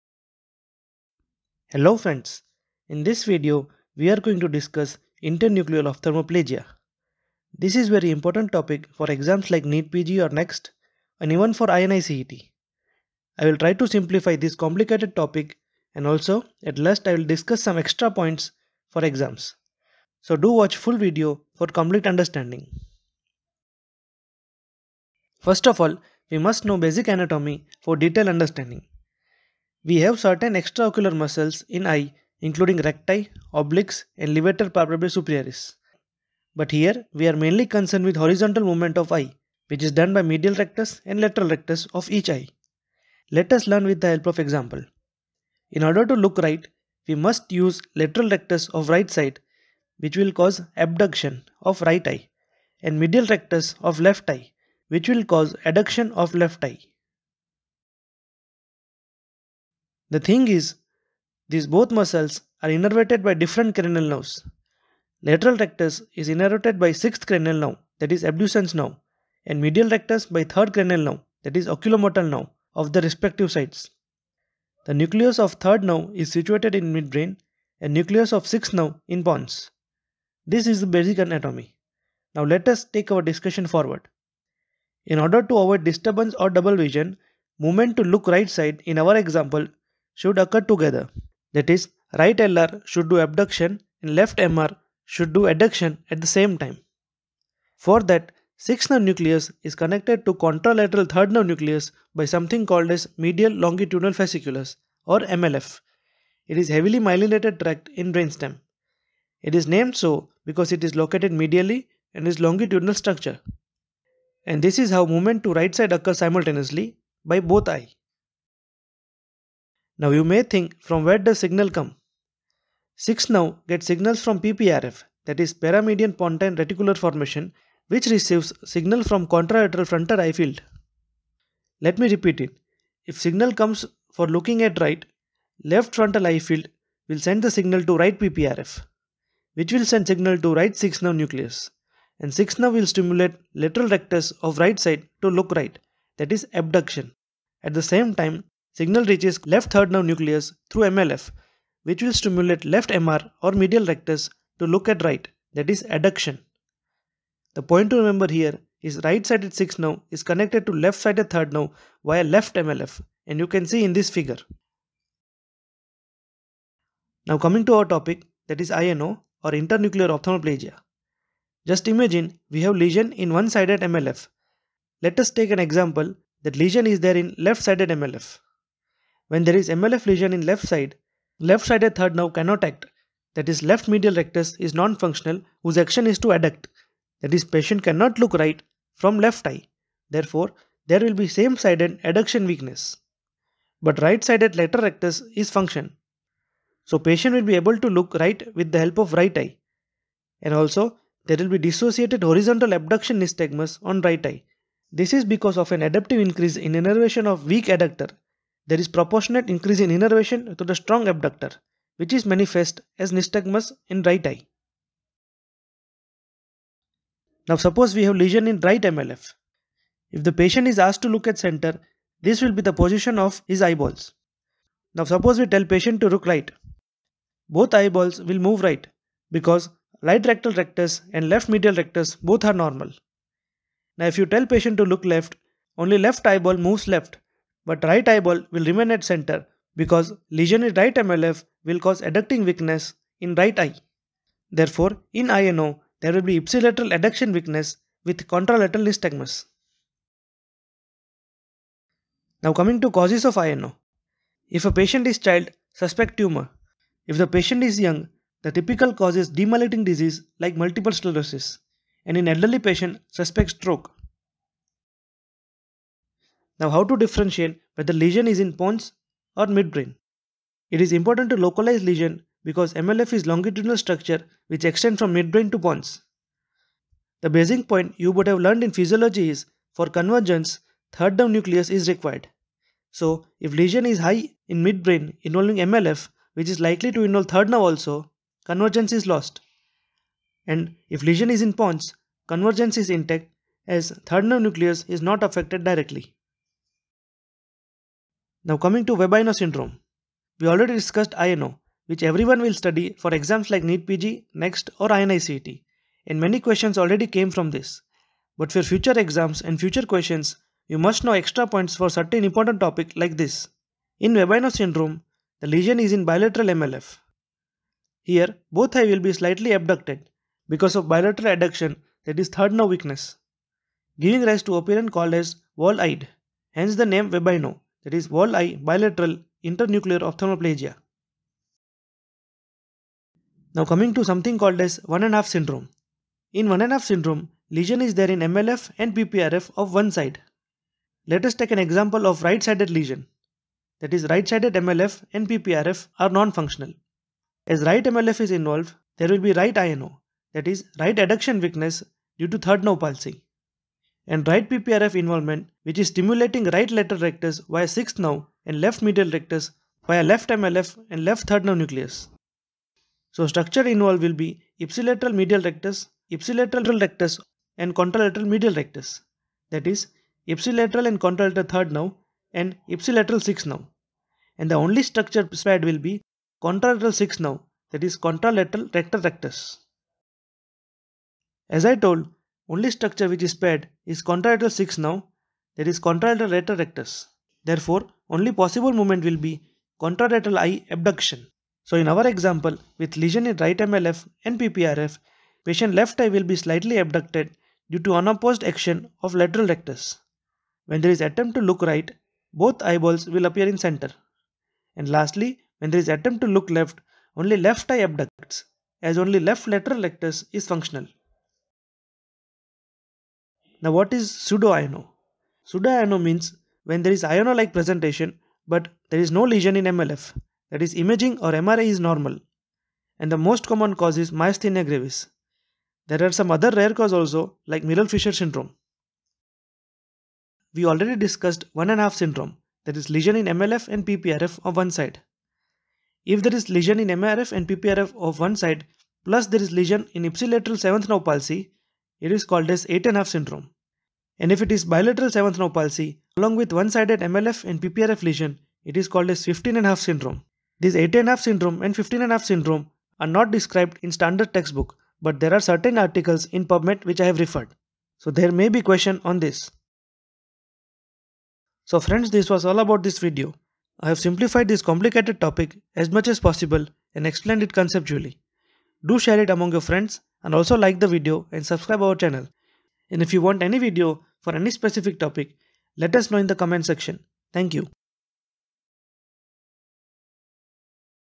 Internuclear Ophthalmoplegia | INO | WEBINO Syndrome | One and Half Syndrome
Hello friends. This video is about Inter-Nuclear Ophthalmoplegia (INO). I have discussed this topic in a simplified way for easy understanding as it is very important topic for exams like NEETPG, NEXT, FMGE and INI-CET.

I have also discussed certain additional important topics like WEBINO Syndrome, Pseudo-INO, One and Half Syndrome, etc.

00:00 : Introduction.
00:25 : Basic Anatomy.
02:46 : INO concept.
03:36 : Lesion in Right MLF.
04:12 : Causes of INO.
04:26 : Mid-Brain v/s Pons Lesion.
04:59 : WEBINO Syndrome.
05:33 : One and Half Syndrome.
06:53 : Pseudo-INO.